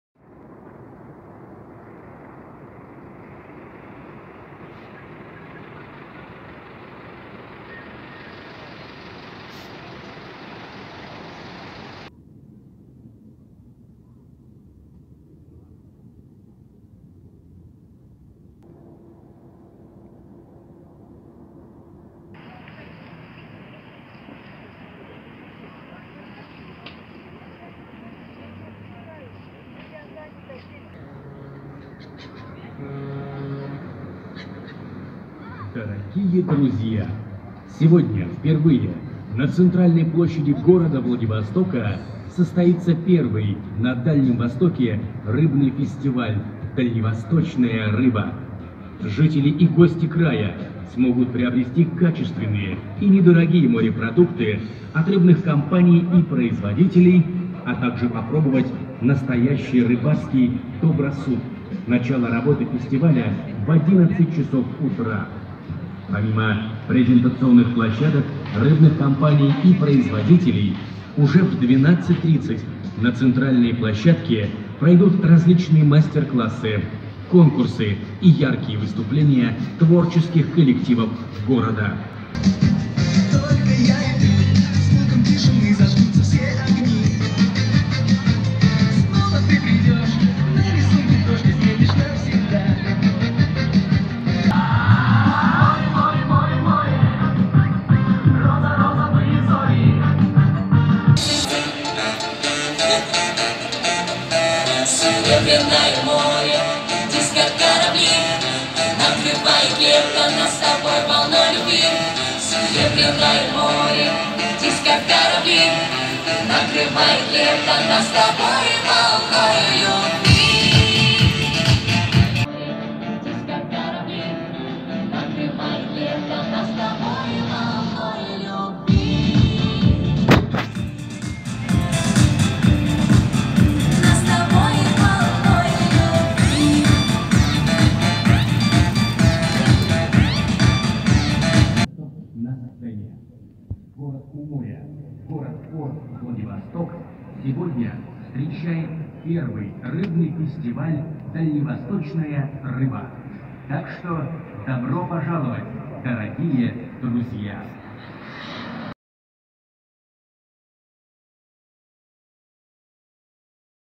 Владивосток Дальневосточная рыба (перед началом фестиваля),
Не знал о этом фестивале,проходил мимо решил снять правда на телефон поэтому такое отвратительное качество.Это ещё фестиваль не начался.